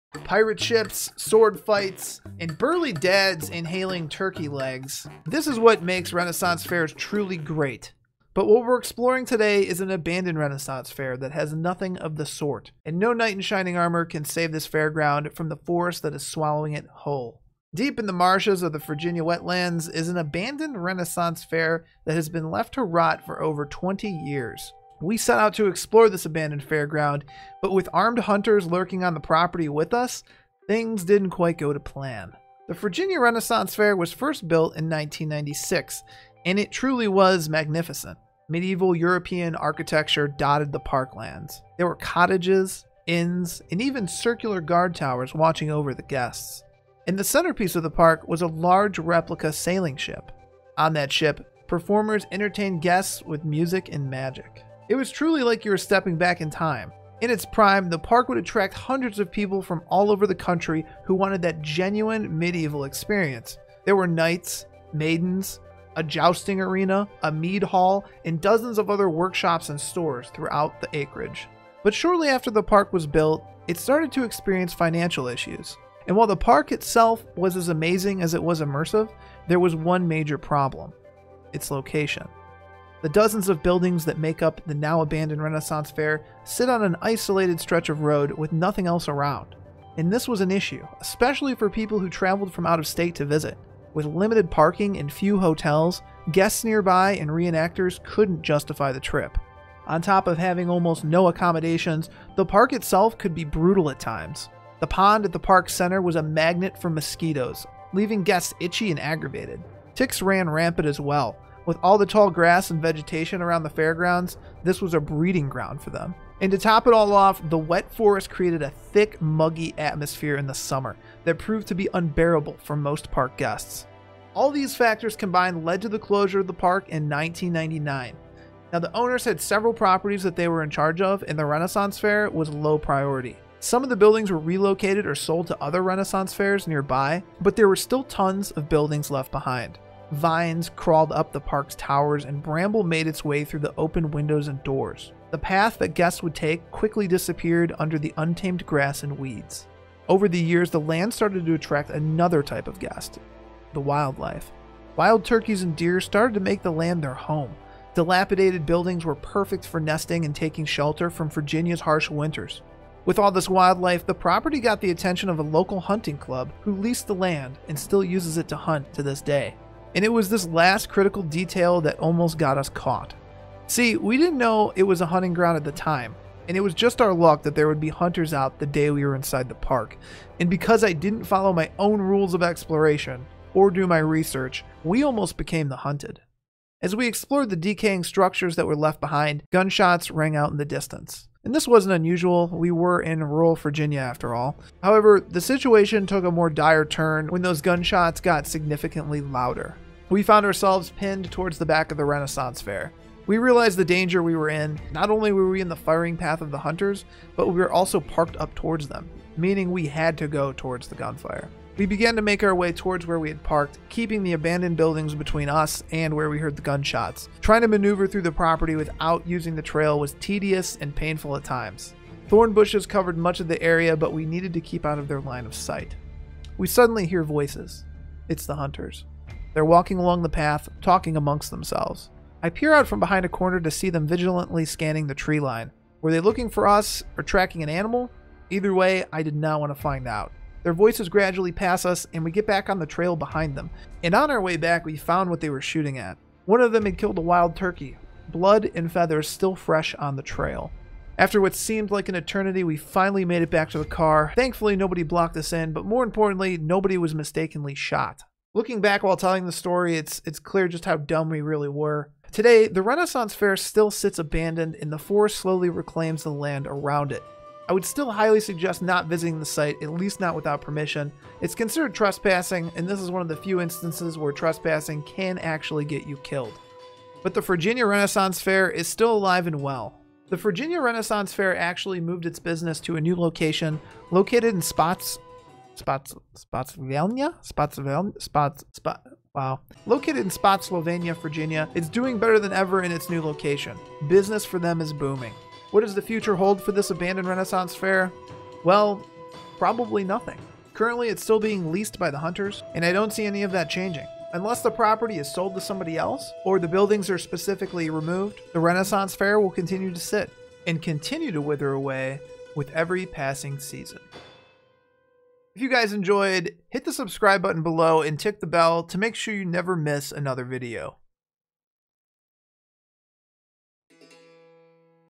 Let's Explore An Abandoned Renaissance Faire
Pirate ships, sword fights, and burly dads inhaling turkey legs. This is what makes renaissance faires truly great.

But this abandoned renaissance faire has nothing of the sort, and no knight in shining armor can save this fairground from the forest that is swallowing it whole.

Deep In the marshes of the Virginia wetlands is an abandoned renaissance faire that has been left to rot for over 20 years.

We set out to explore the abandoned fairground, but with armed hunters lurking on the property with us.. Things didn’t quite go to plan..

The Virginia Renaissance Faire was first built in 1996, and it was magnificent. Medieval European architecture dotted the parkland.

There were cottages, inns, and even circular guard towers watching over the guests.

The centerpiece of the park was a large replica sailing ship. On the ship performers entertained guests with music and magic.

In its prime, the park would attract hundreds of people from all over the country who wanted a genuine medieval experience. There was a jousting arena, mead hall, and dozens of other workshops and stores.

But shortly after the park was built it started to experience money problems. While the park itself was as amazing as it was immersive, there was one major problem. It’s location.

The dozens of buildings that make up the now abandoned renaissance faire sit on an isolated stretch of road with nothing else around.

This was an issue, especially for people who traveled from out of state to visit. With limited parking, and few hotels nearby guests and renactors couldn’t justify the trip.

On top of having almost no accommodations, the park itself could be brutal at times. The pond at the parks center was a magnet for mosquitoes, leaving guests itchy and aggravated.

Ticks ran rampant as well, with all the tall grass and vegetation around the fairgrounds. And to top it off, the wet forest created a thick muggy atmosphere in the summer, that proved to be unbearable for most park guests.

All of these factors combined led to the closure of the park in 1999. The owners had several properties that had priority over the renaissance faire, so it was left for nature to reclaim.

Some of the buildings had been relocated or sold to other renaissance faires, but there were still tons of buildings left behind.

The forest around the abandoned renaissance faire began to swallow buildings whole. Vines crawled up the parks towers and bramble made its way through open windows and doors.

The paths that guests would take quickly disappeared under untamed grass and weeds.

Over the years the land started to attract another type of guest. The wildlife. Wild turkeys and deer started to make the land their home.

Dilapidated buildings were perfect for nesting, or taking shelter from Virginia’s harsh winters. The property got the attention of a local hunting club, who purchased the land still uses it to hunt game to this day.

And it was that last critical detail that almost got us caught. See, we didn’t know it was a hunting ground at the time. And it was just our luck that there would be hunters out the day we were inside the park. And because I didn’t follow my own rules of exploration, I almost became the hunted.

As we explored the decaying structures that were left behind, gunshots rang out in the distance. This wasn’t unusual, we were in rural Virginia after all. However the situation took a more dire turn when those gunshots suddenly got significantly louder.

Pinned towards the back of the abandoned renaissance faire, we realized the danger we were in. Not only were we in the firing path of the hunters, but we were also parked up towards them.

We began to make our way towards where we parked, keeping the abandoned buildings between us and the where we heard the gunshots.

Trying to maneuver through the property without using the trail was tedious and at times painful. Thorn bushes cover much of the area, but we needed to keep out of their line of sight.

We suddenly hear voices, it’s the hunters. They’re walking along the path talking among themselves, I peer out from behind a corner to see them vigilantly scanning the treeline and buildings.

Were they looking for us, or just tracking an animal? Either way, I didn’t want to find out.

Their voices gradually pass us and we get back on the trail behind them. On our way back we found what they were shooting at. One of them had killed a wild turkey, blood and feathers still fresh on the trail.

After what seemed like an eternity, we finally made it to the car. Thankfully no one blocked us in, but more importantly no one was mistakenly shot.

Today the renaissance faire still sits abandoned, and the forest slowly reclaims the land around it.

Thanks to Urbex And Chill, Drone Delegation, and Wicked Good Productions for additional footage and images.